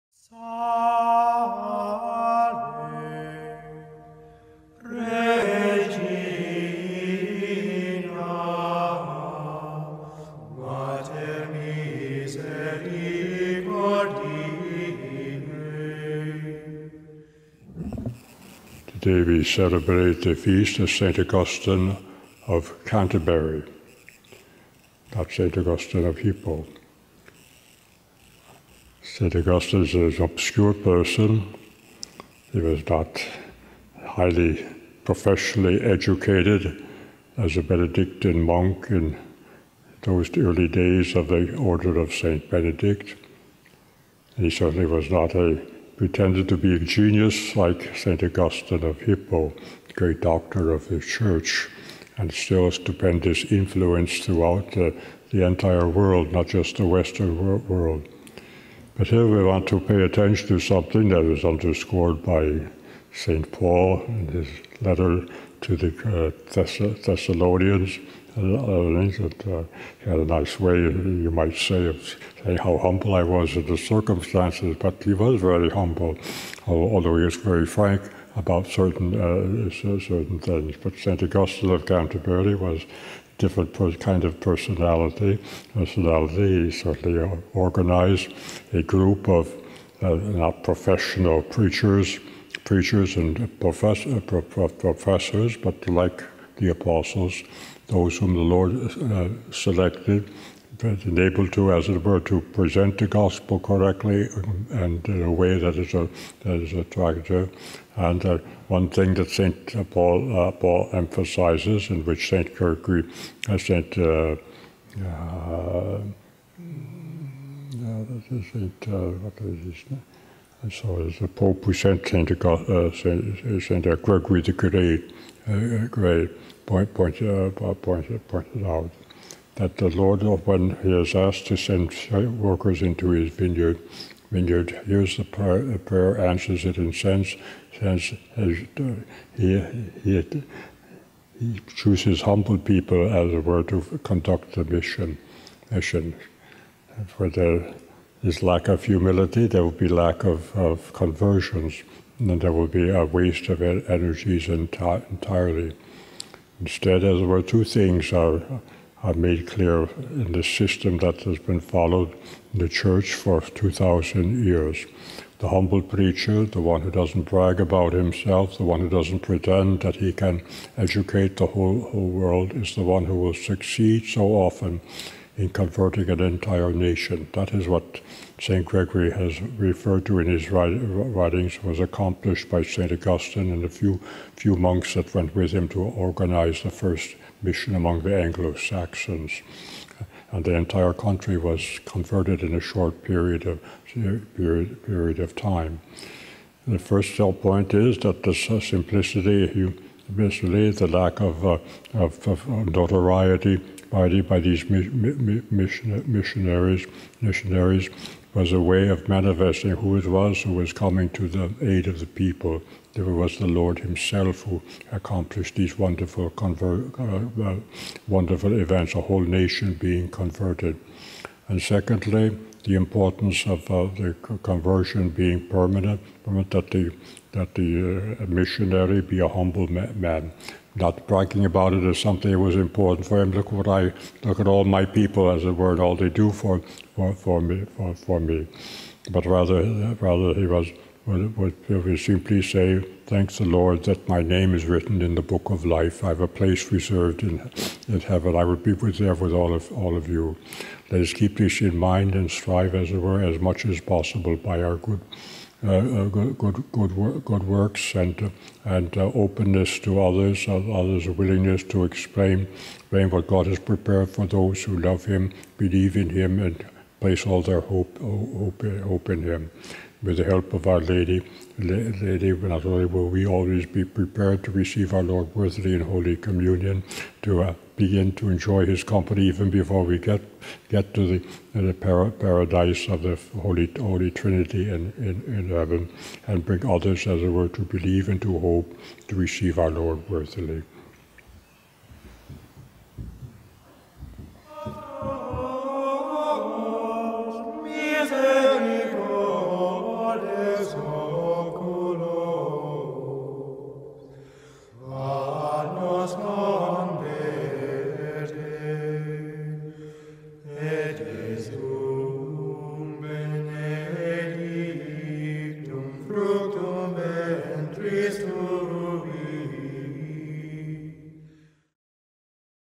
May 27 - Homily - Fr Peter: Humble Missionaries
Humble Missionaries
As missionaries of Christ, we all must strive for humility. St. Augustine of Canterbury, sent by Pope St. Gregory the Great to England, was such an example.
Ave Maria!

Mass: St. Augustine of Canterbury - Opt Mem
Readings: Friday 8th Week of Ordinary Time
1st: 1pe 4:7-13
Resp: psa 96:10, 11-12, 13 0